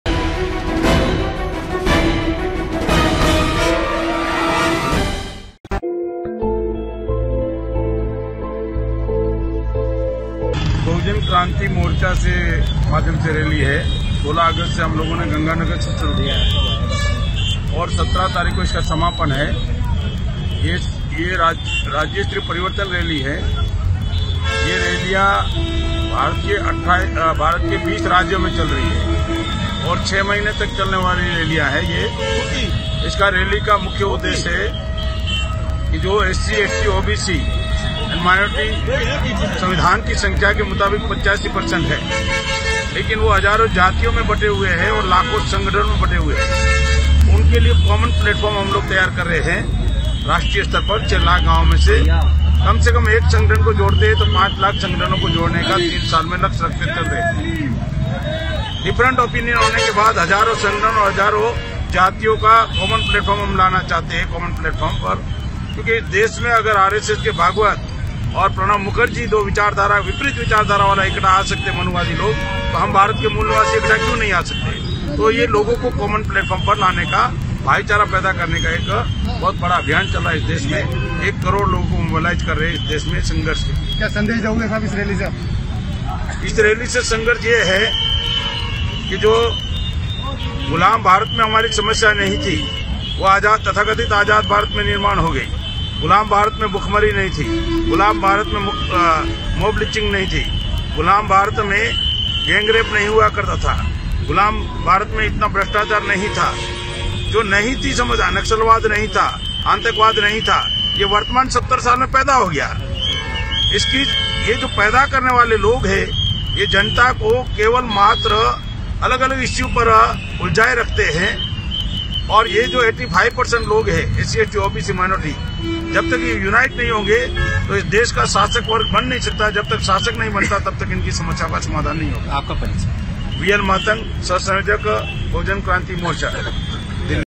बहुजन क्रांति मोर्चा के परिवर्तन यात्रा ने बयां की क्रांतिकारी भूमिका
बहुजन क्रांति मोर्चा के राष्ट्रिय परिवर्तन यात्रा के राष्ट्रिय सह-संयोजक वि.एल. मातंग जी ने राजस्थान, अजमेर में बयां की क्रांतिकारी भूमिका